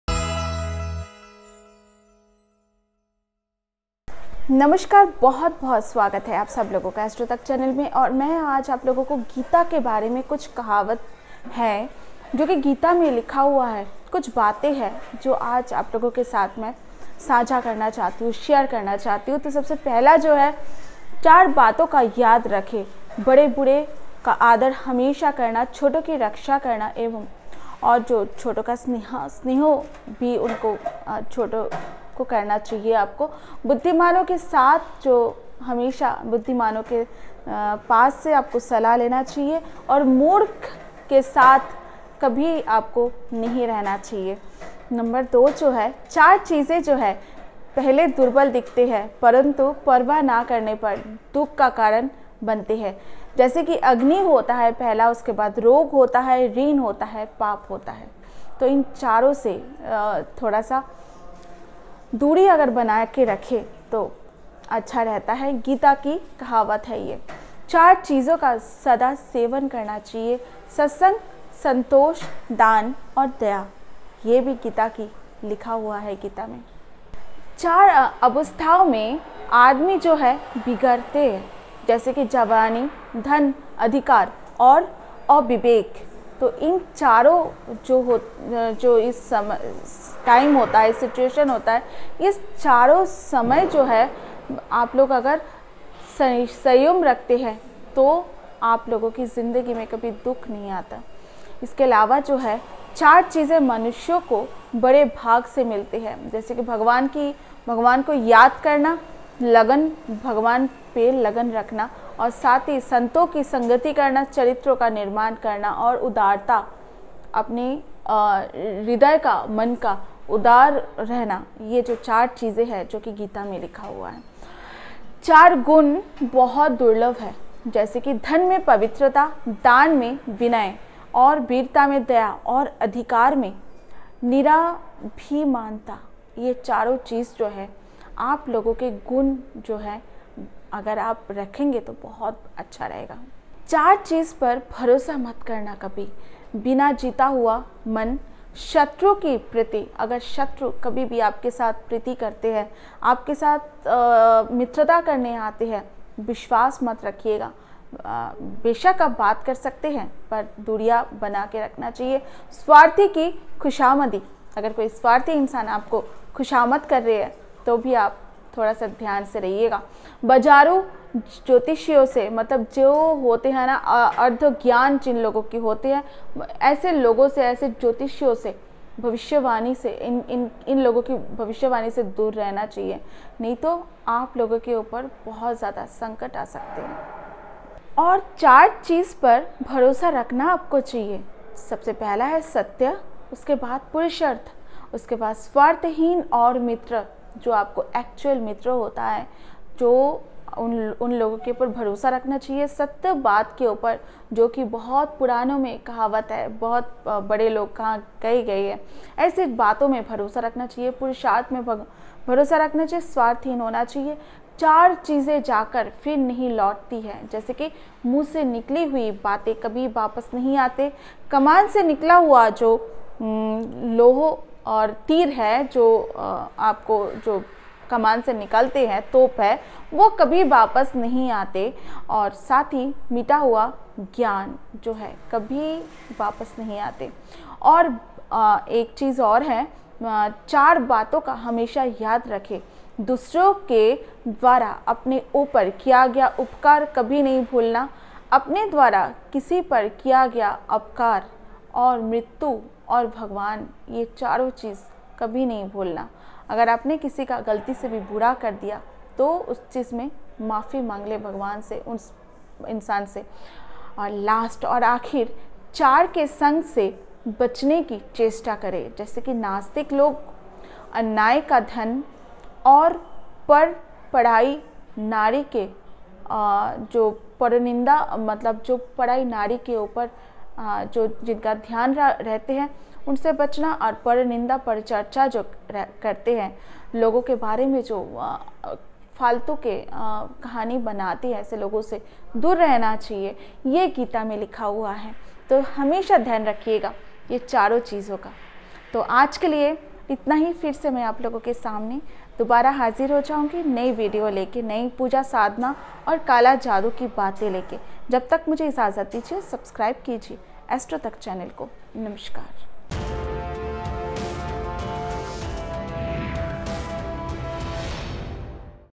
भागवत गीता की इन 5 बातों का रखें ध्यान | Pritika Majumdaar | Astro Tak
ज्योतिर्विद प्रीतिका मजूमदार से काला जादू में जानिए भागवत गीता की किन 5 बातों का खास ध्यान ध्यान रखना होता है जरूरी ?